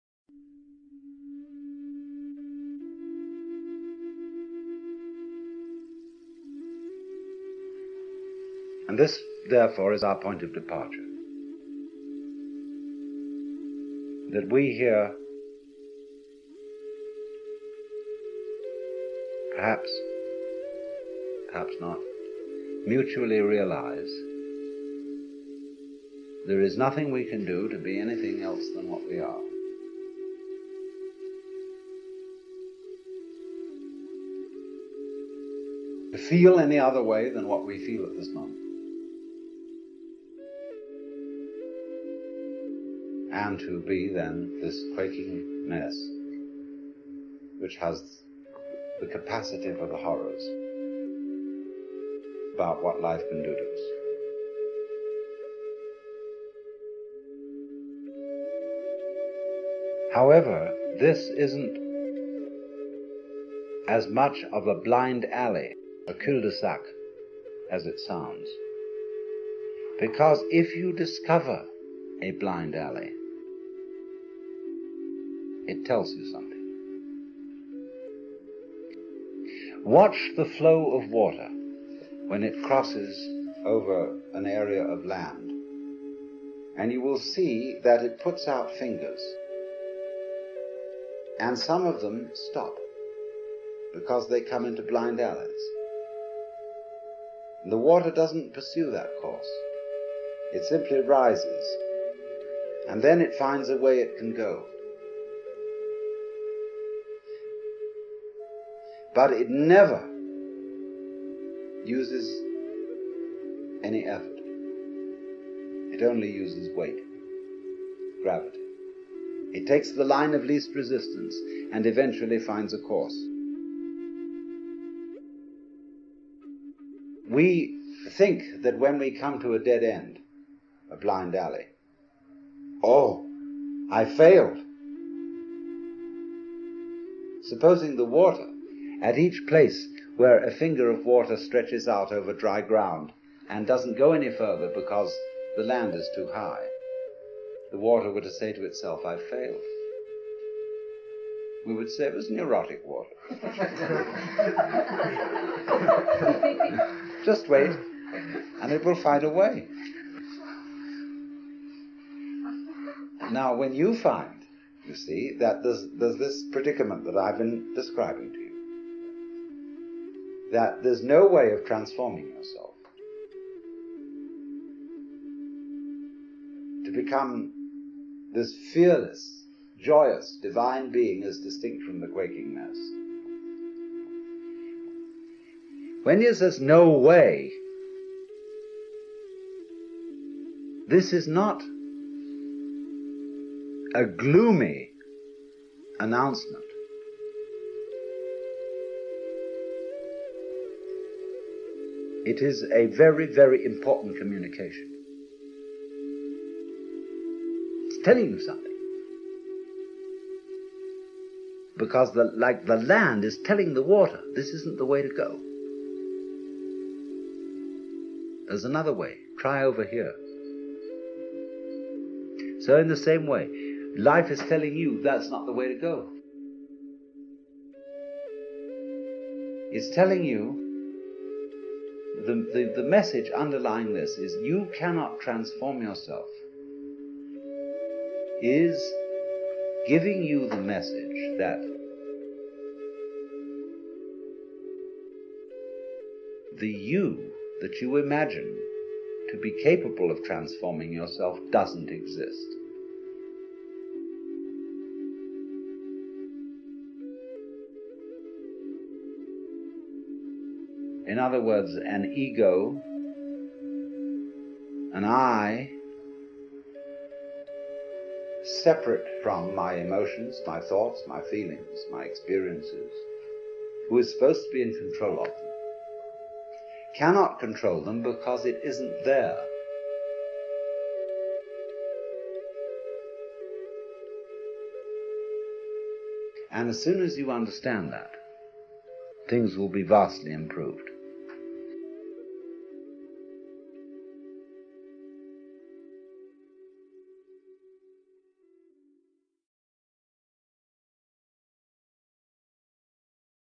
Alan Watts - Flow
Alan Wilson Watts was a British writer and speaker known for interpreting and popularising Buddhism, Taoism, and Hinduism for a Western audience. Born in Chislehurst, England, he moved to the United States in 1938 and began Zen training in New York

Thanks For Waching Peace And Love Everyone !